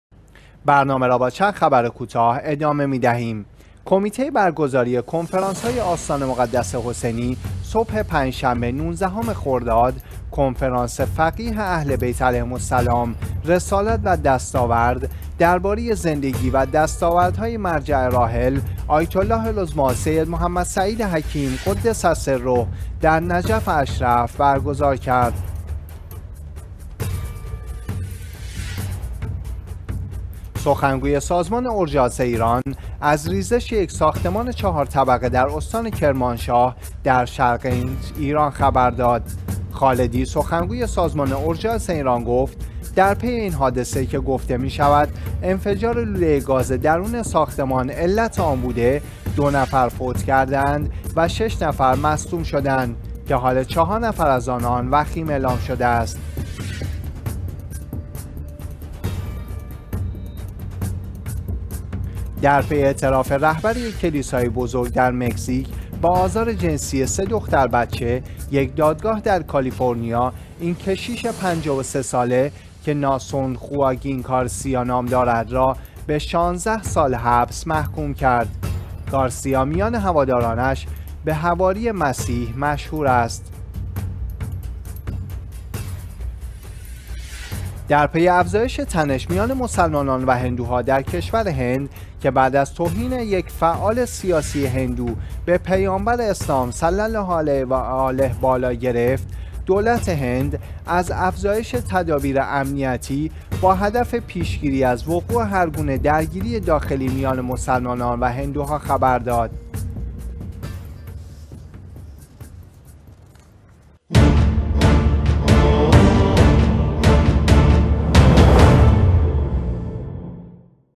نگاهی به چند خبر کوتاه:
۱)برگزاری کنفرانس «فقیه اهل بیت علیهم السلام رسالت و دستاورد» در نجف اشرف
۲)ریزش ساختمان ۴ طبقه در ایران استان کرمانشاه
۳)۱۶ سال حبس برای "حواری مسیح" در پی اعتراف به آزار جنسی ۳ دختر بچه
۴)افزایش تدابیر امنیتی در هند